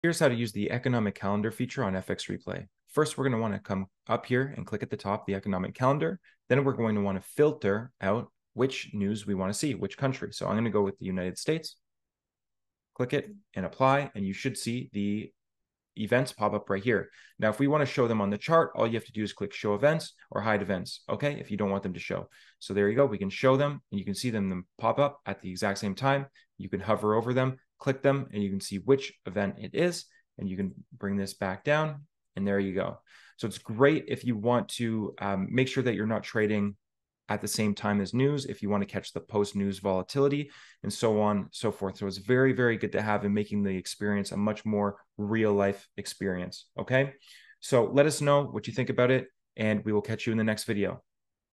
How to use the Economic Calendar feature on FX Replay
Here's a quick video on how to use the economic calendar feature.